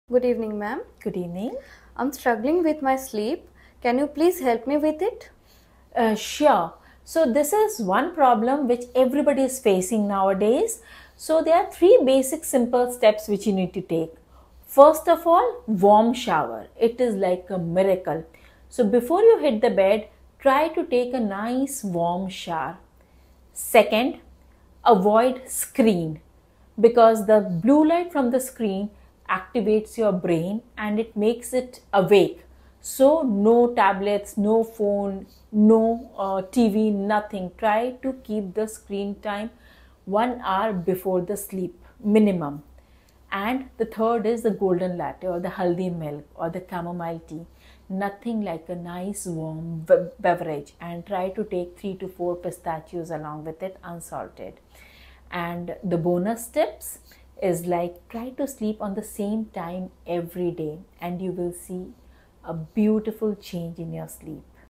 Tips for Sleeping Issues || Fight Insomnia
Going to bed and waking up at the same time every day can also significantly improve your sleep quality.

While you might be tempted to sleep in on the weekends, this can make it more difficult to wake up and fall asleep during the workweek.
Register with us now and see the change.
To register connect on - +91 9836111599